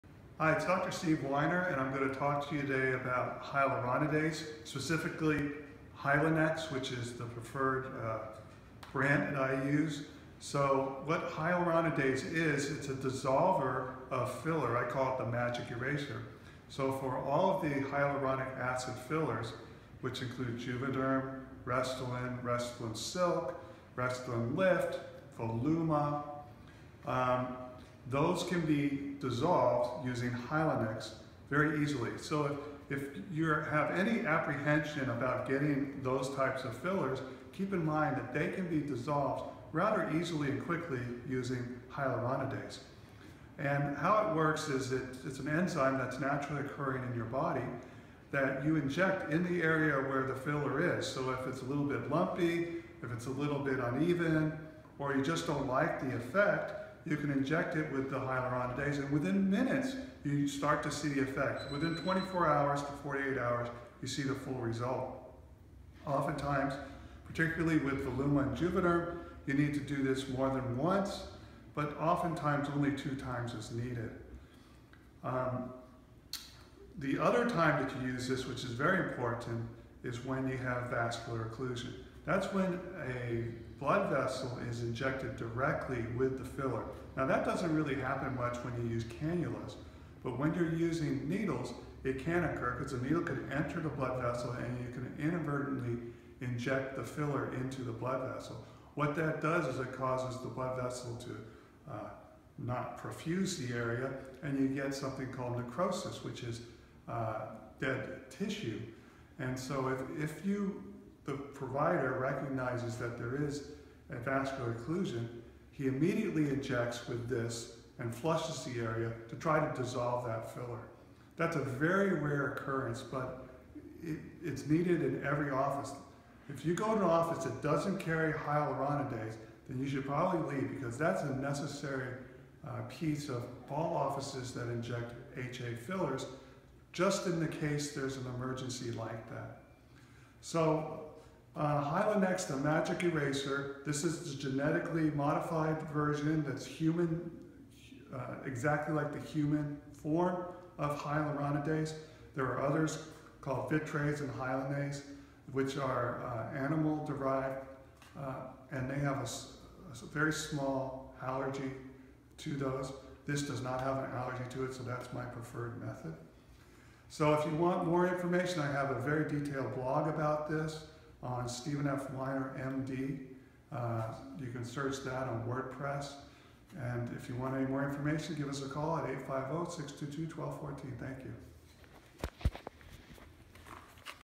Hyaluronidase for Erasing Unwanted/Bad Hyaluronic Acid (Juvederm/Restylane). Dr Weiner Expert.
Hyaluronidase is used to dissolve any Hyaluronic Acid filler. It is injected into the area that is desired to be removed. It can be used for poorly placed filler, poor cosmetic results, lumpy filler, or in an emergency situation where there is vascular occlusion. It often requires more than one treatment.